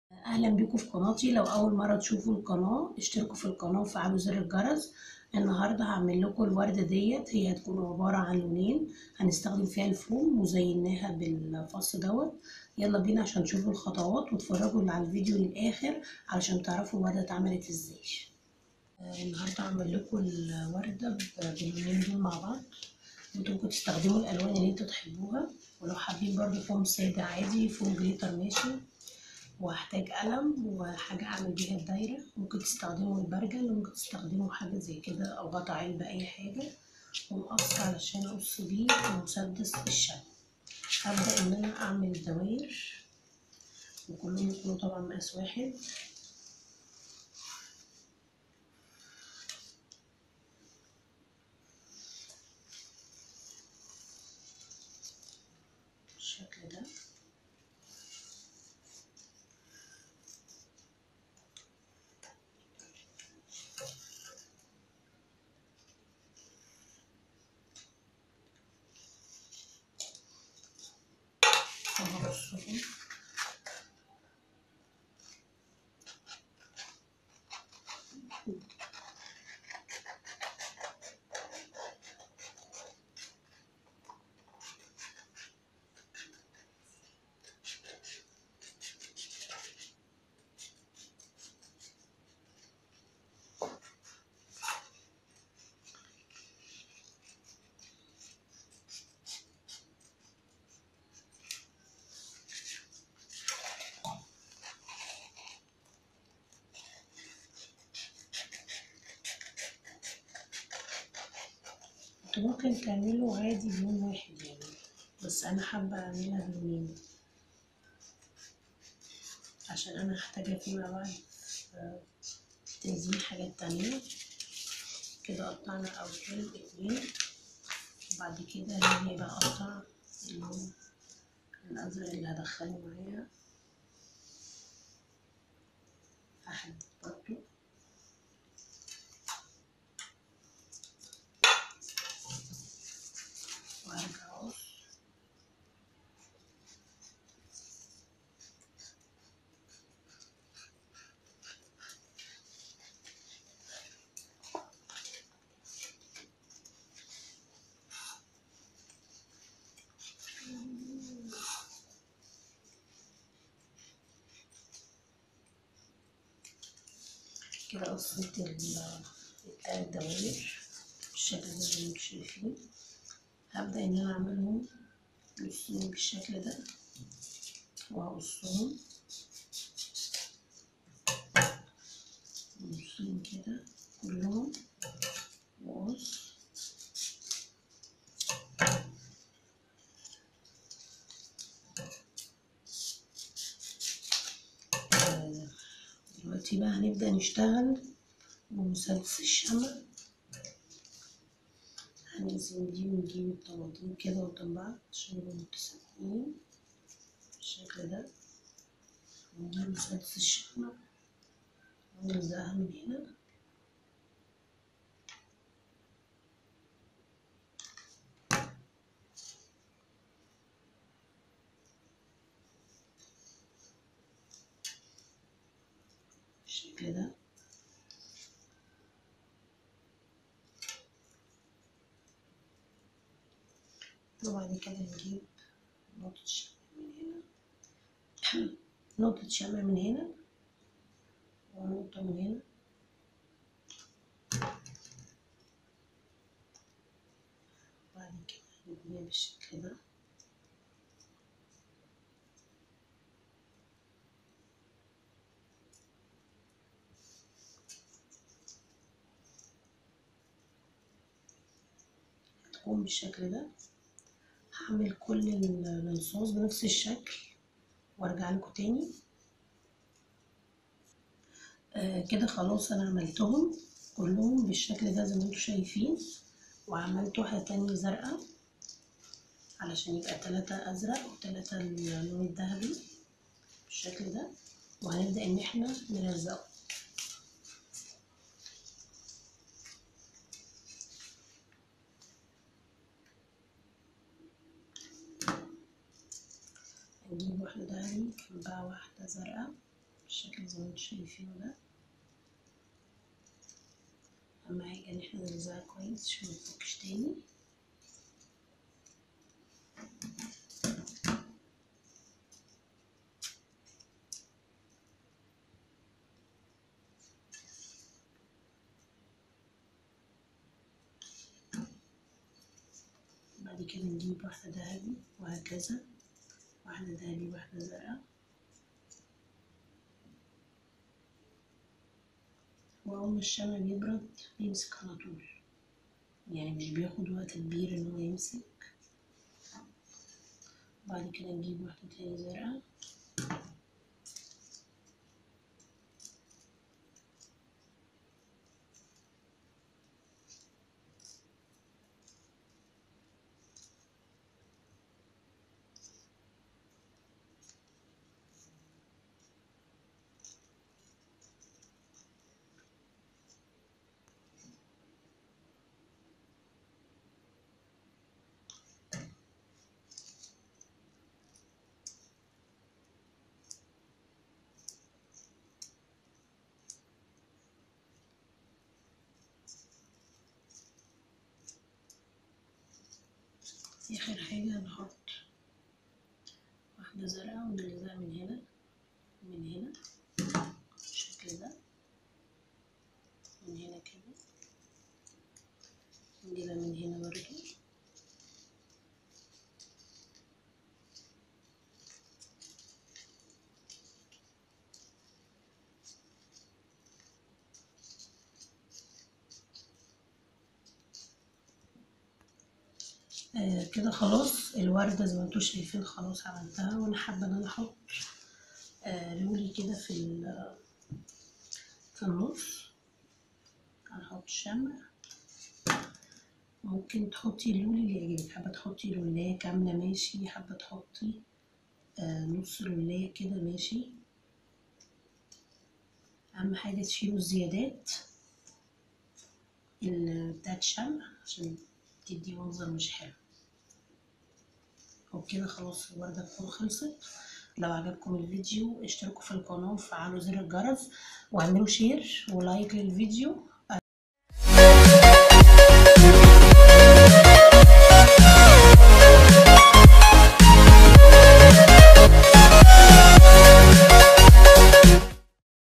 طريقة عمل وردة الفوم بطريقة سهلة جدااااا
اعملي احلي وردة من الفوم بطريقة سهلة جداااااا
وزيني بيتك وكتبك وادواتك باحلي اشكال من الفوم